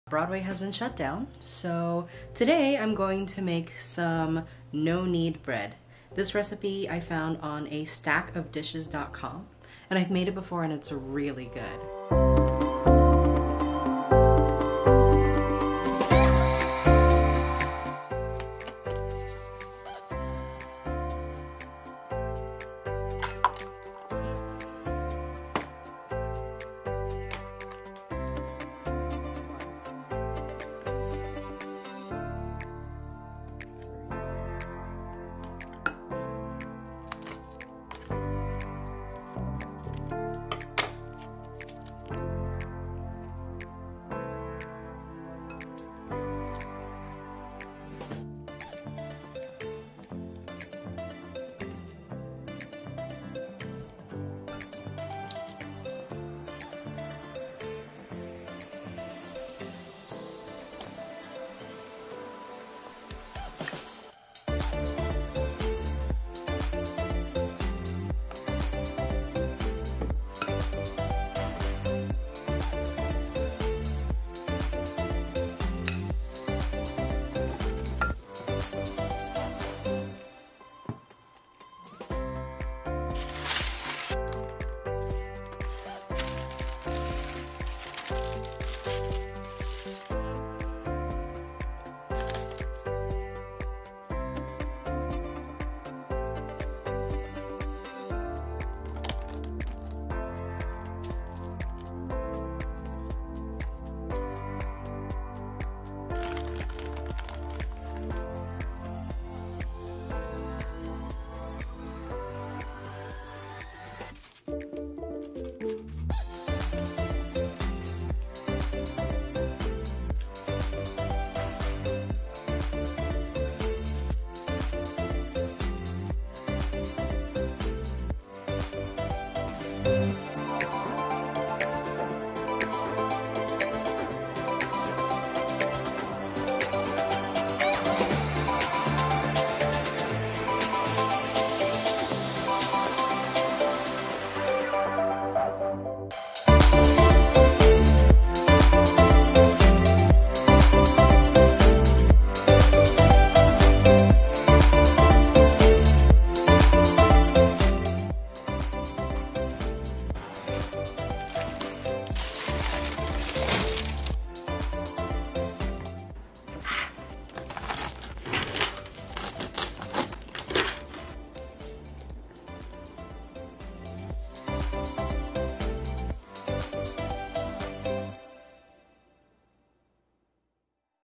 Simple No Knead Bread recipe.
Made a basil and 5 cheese loaf.